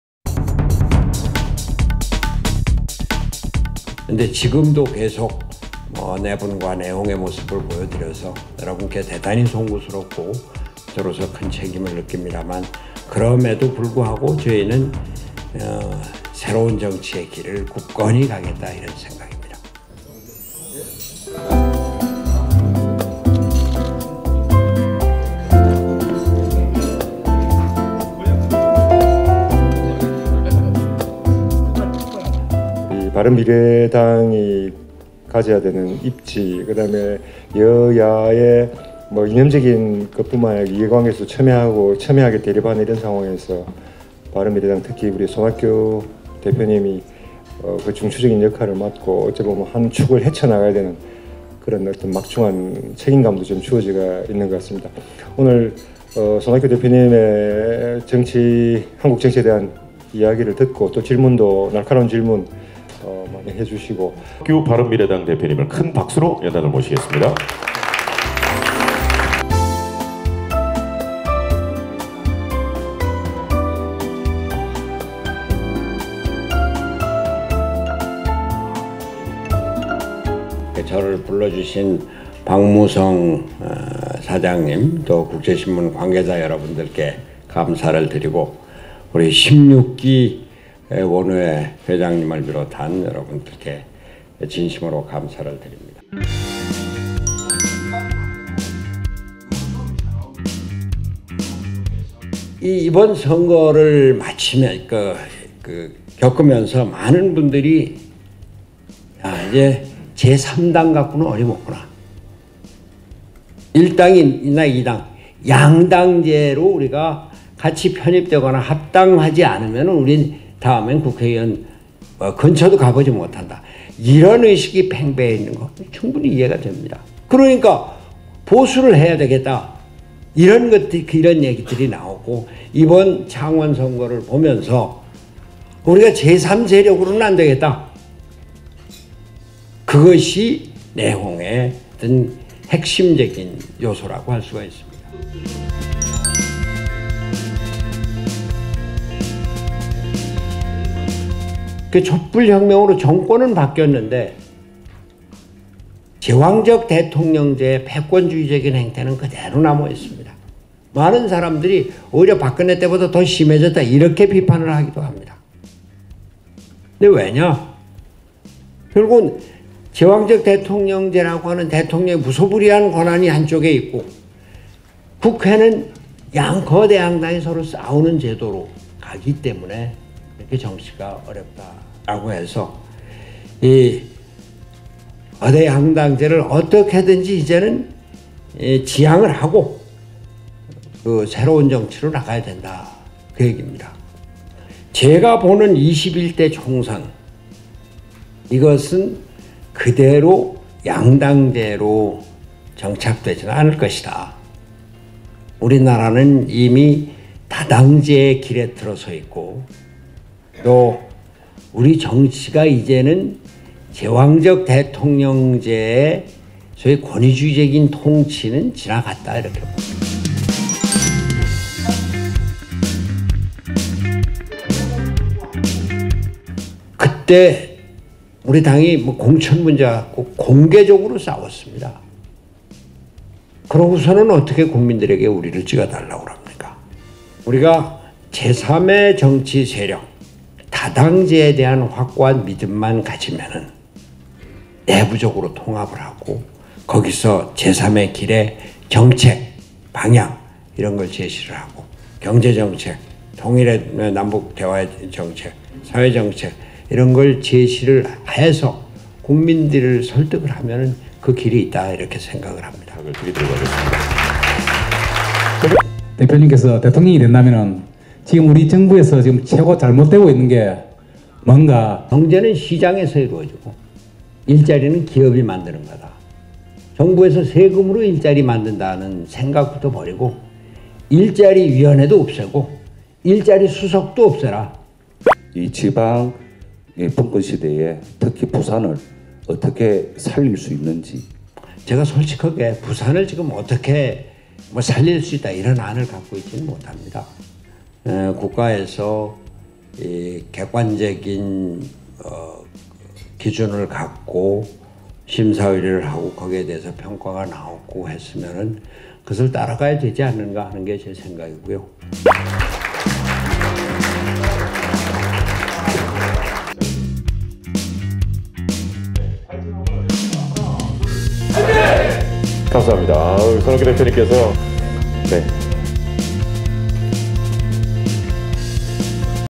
손학규 대표가 말하는 바른미래당 장밋빛 미래
17일 부산롯데호텔에서 열린 제16기 국제아카데미 7주 차 강연자로 나선 바른미래당 손학규 대표는 ‘내년 총선을 말하다’는 주제로 강연을 했다.